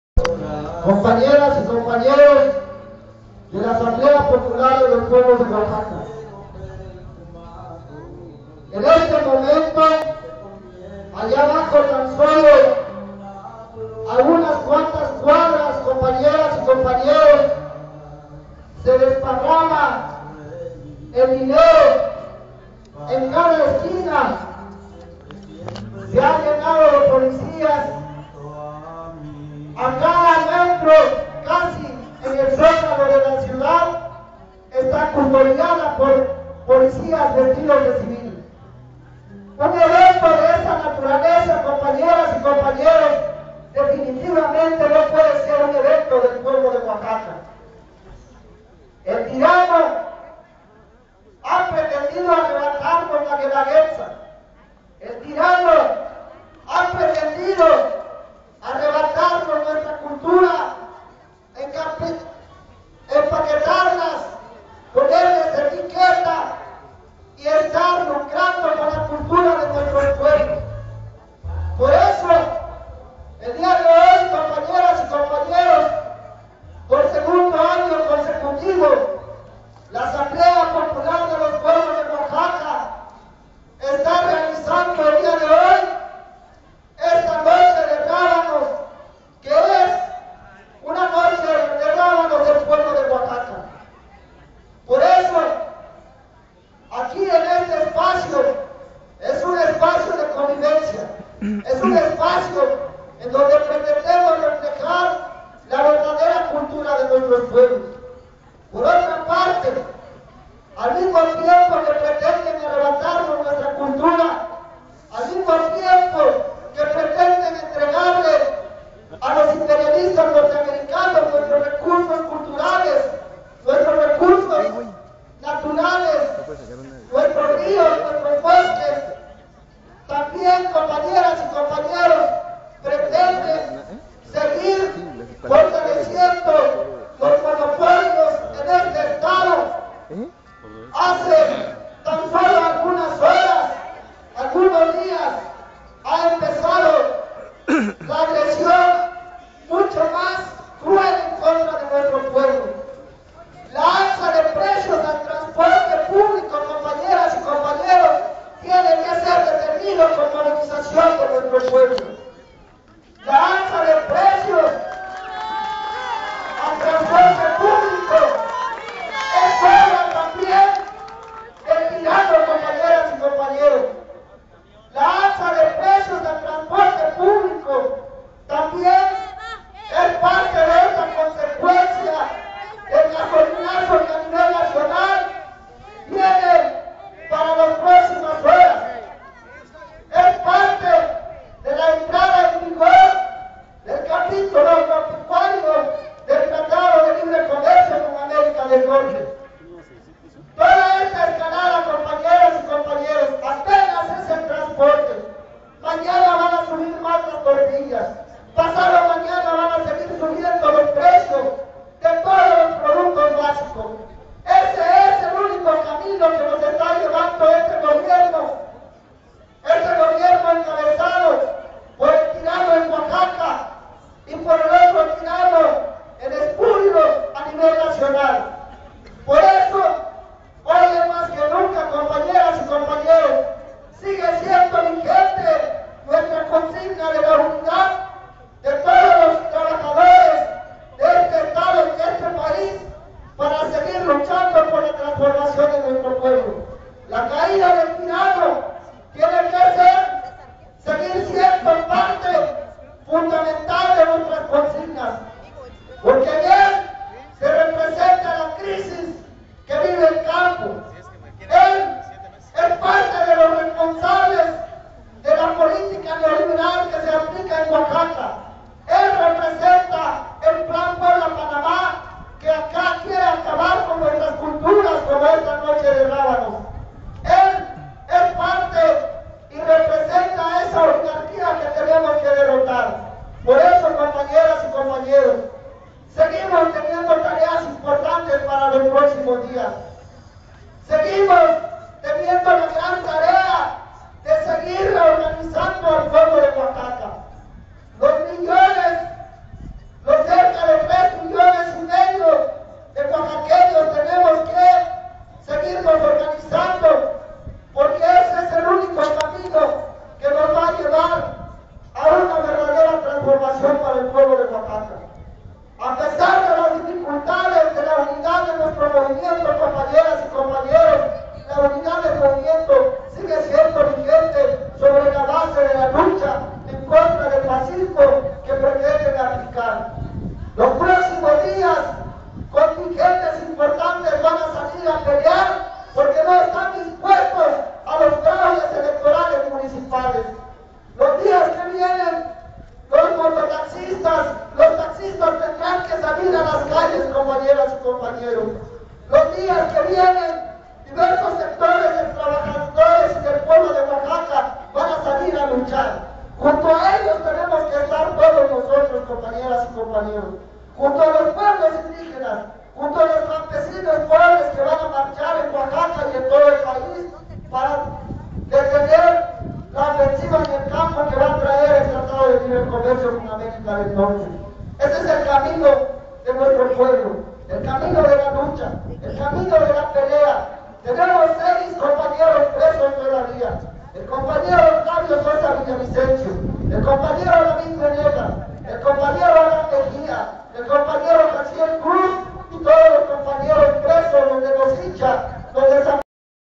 Llama Florentino López a organizarse contra las alzas
Uno de los voceros de la appo llamó a la organización para frenar las alzas en todos los frnetes.